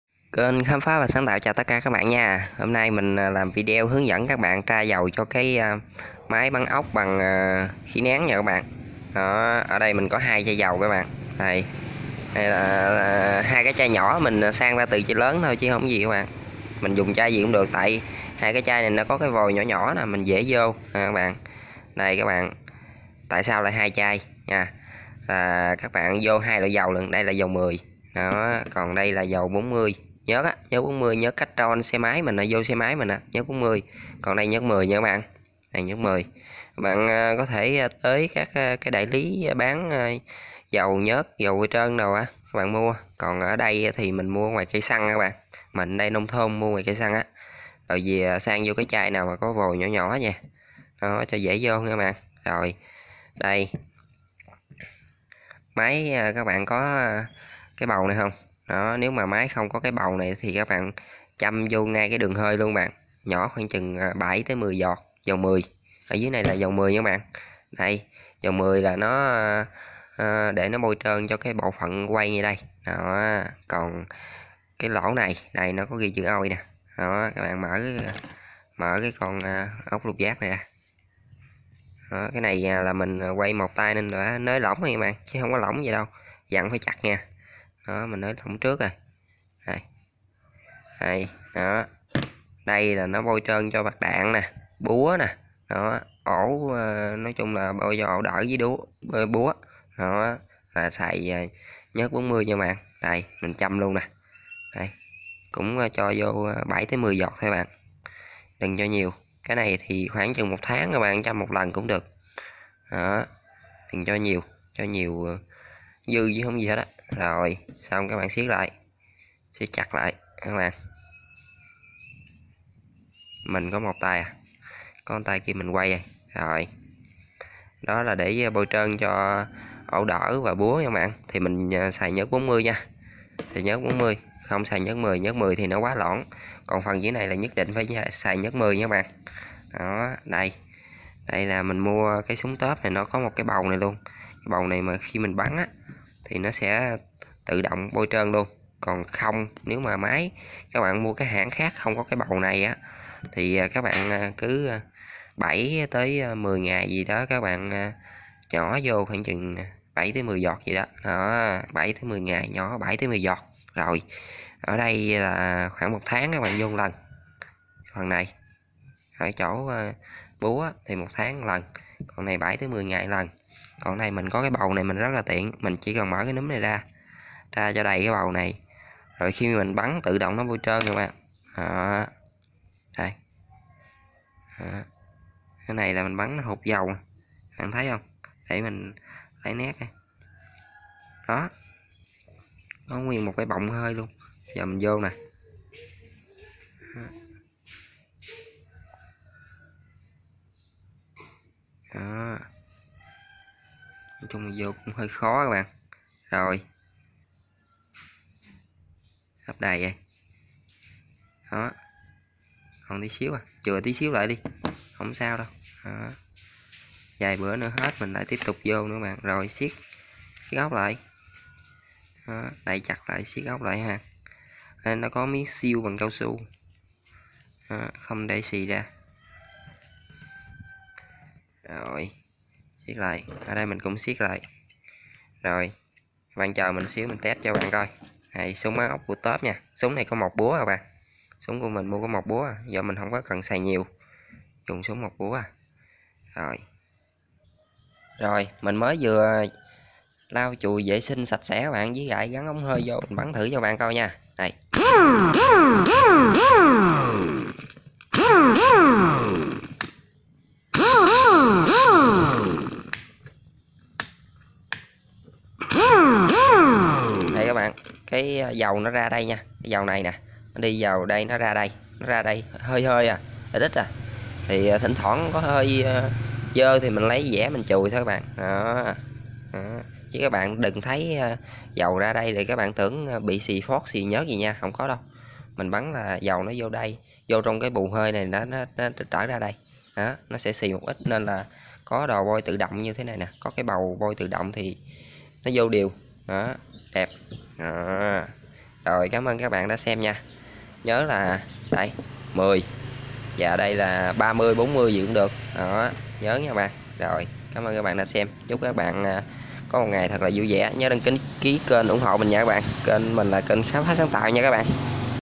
Tra dầu cho súng bắn ốc khí nén
hướng dẫn các bạn cách tra dầu bôi trơn cho máy bắn ốc khí nén, dùng dầu gì là phù hợp, tra bao nhiêu là đủ, và bao lâu thì tra 1 lần